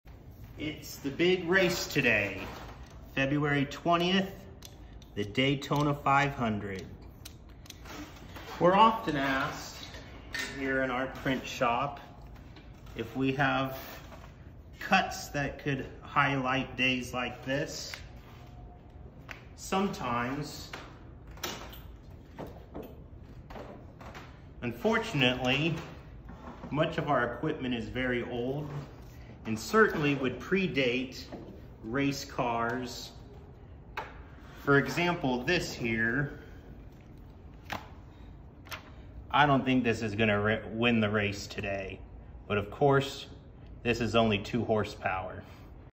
Printing an image from a cut for the Daytona 500
It’s the big race today! In honor of the Daytona 500, Jared printed an image from a cut of something that would definitely come in last place in today’s race.